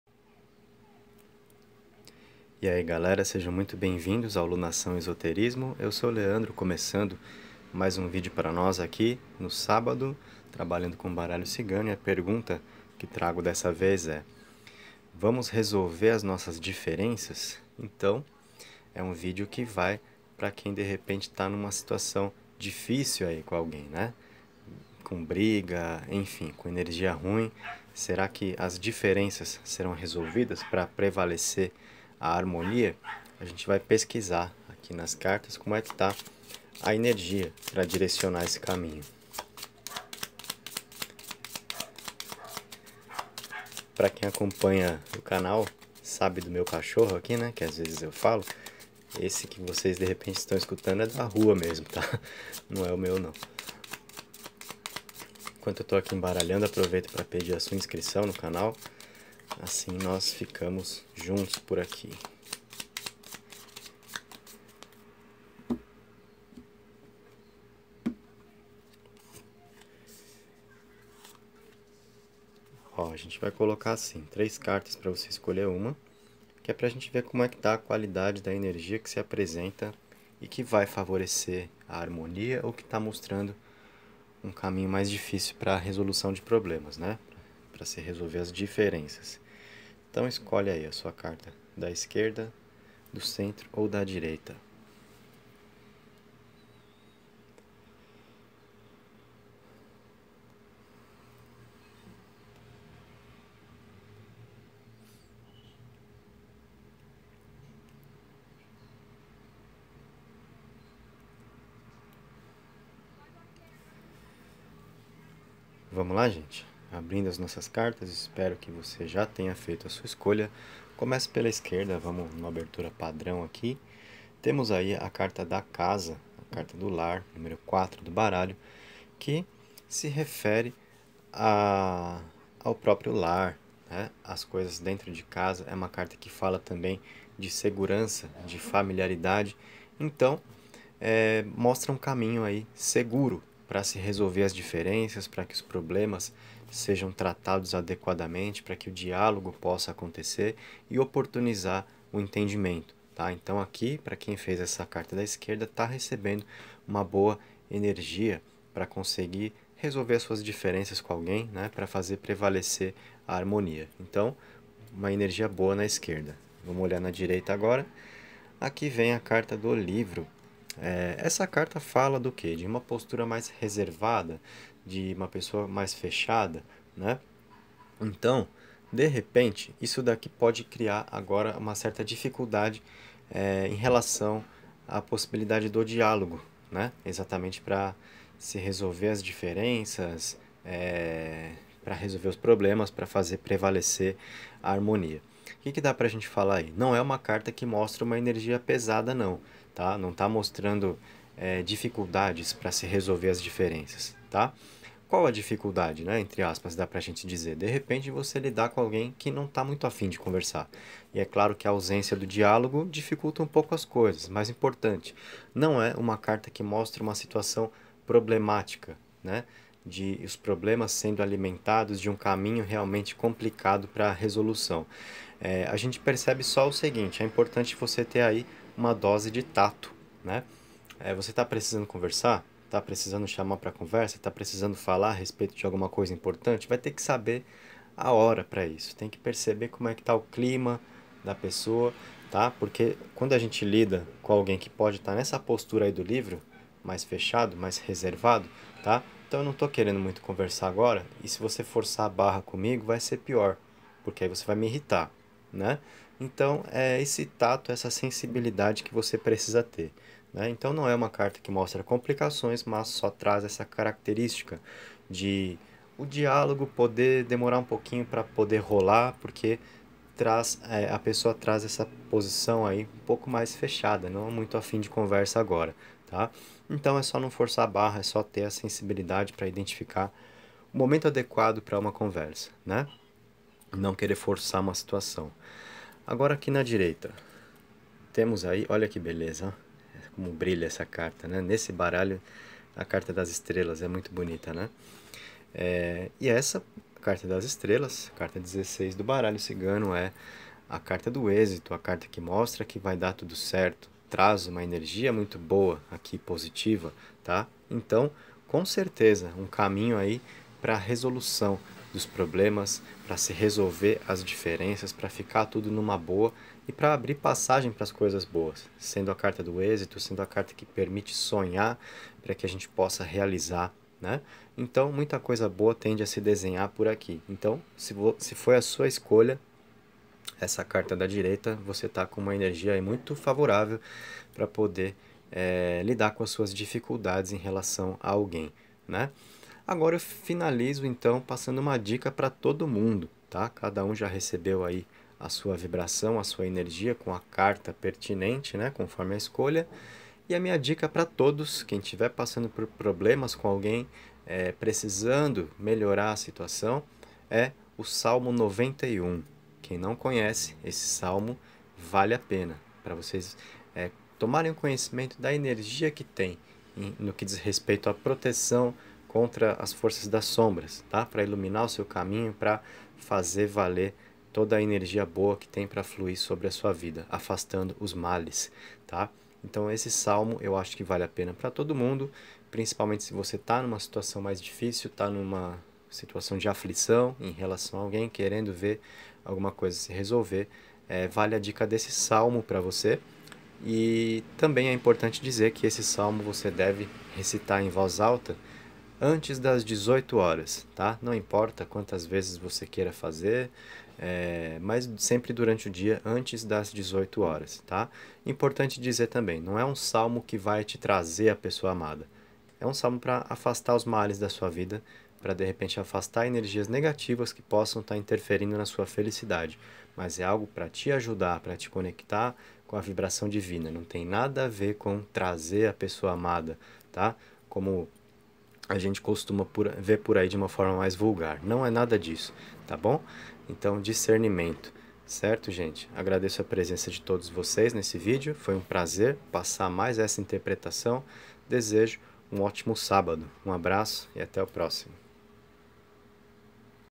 Vamos resolver nossas diferenças?
Tiragem de cartas do baralho cigano para te ajudar a entender se a harmonia poderá prevalecer.